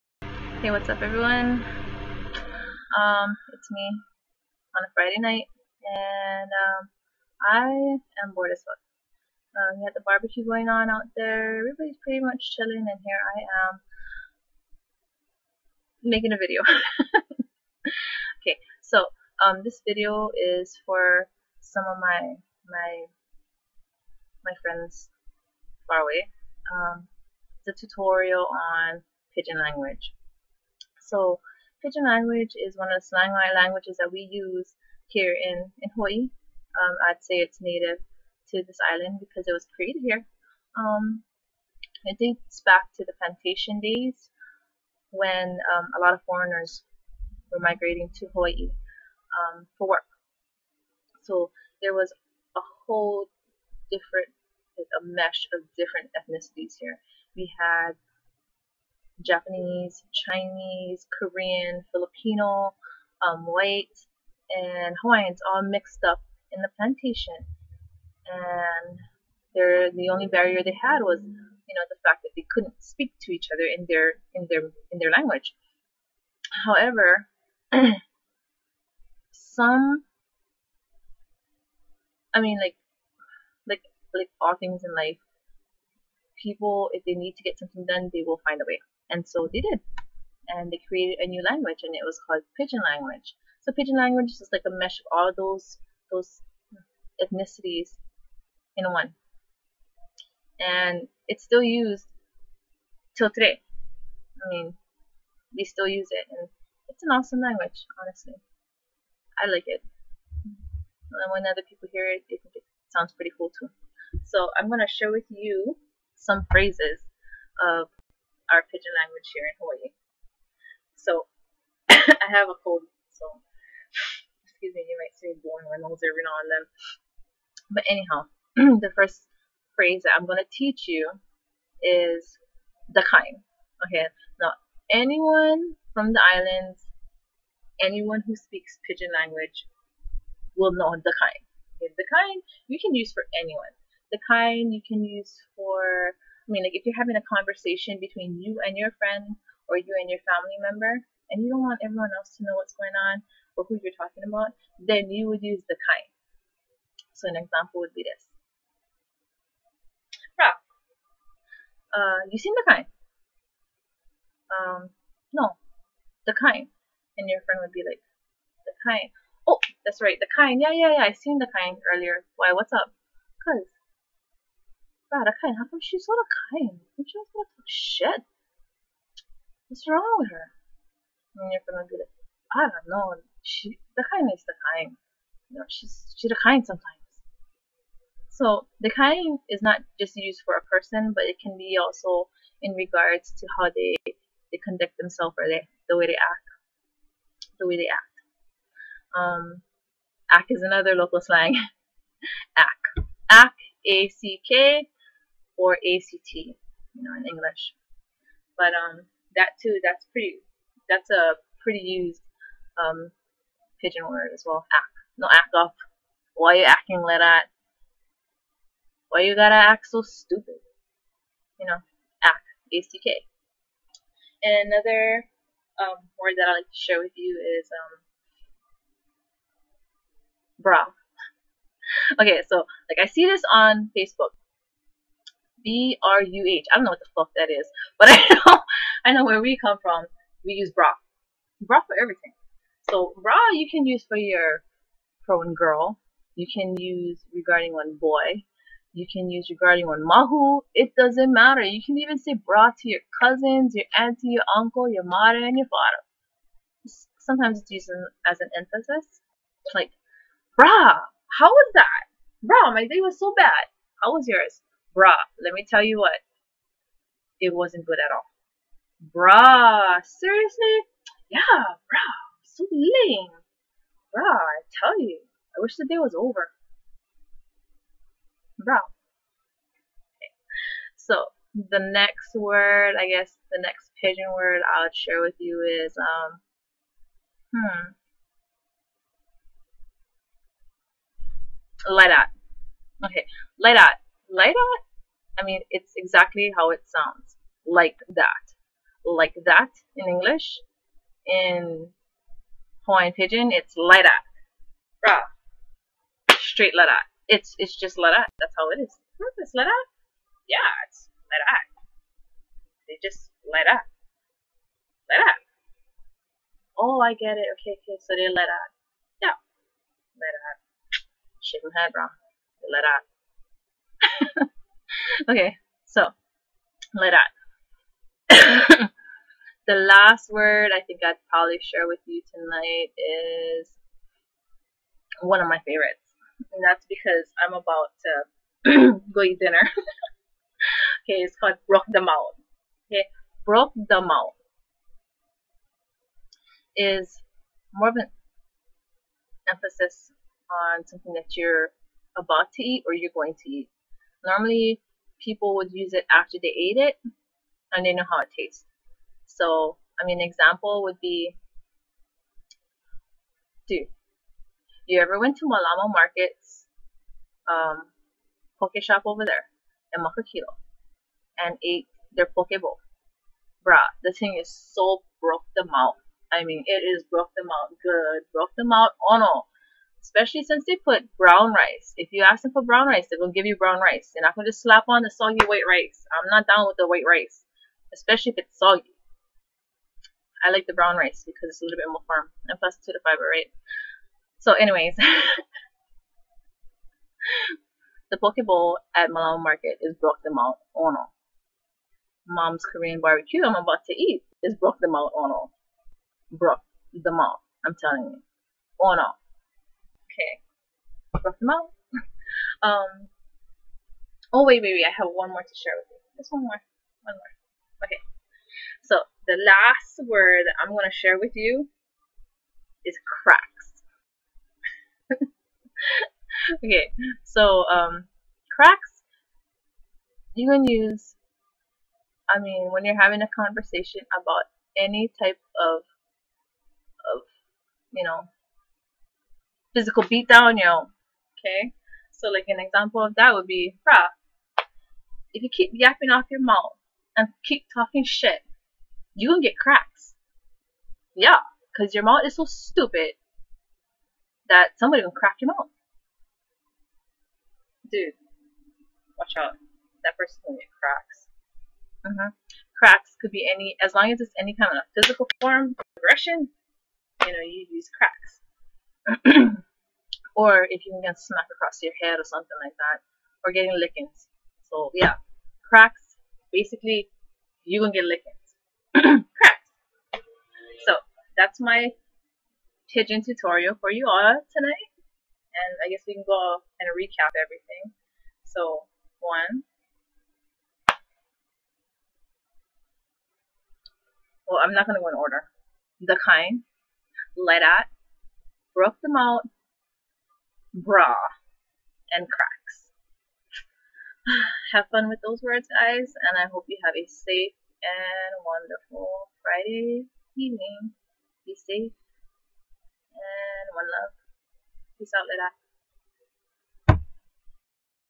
Local Hawaiian Pidgin Language 101
Yes.. I am from Hawai'i born and raised and we do have our own language.  By law the legal spoken language is English and Hawaiian but we locals have our own way of communicating to one another.  Here are the basics of Hawaiian pidgin language with a twist of local humor.  Enjoy!